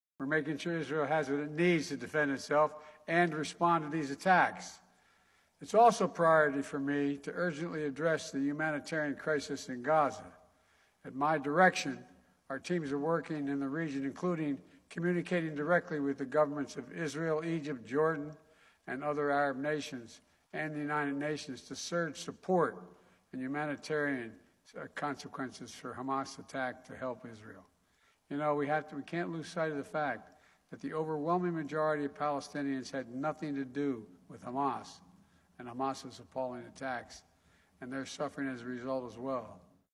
'Humanitarian crisis' in Gaza, warns Biden
Joe Biden said it was a 'priority' to 'address the humanitarian crisis in Gaza'

It comes as the situation in Gaza reached "a dangerous new low", according to the UN Secretary General Antonio Guterres.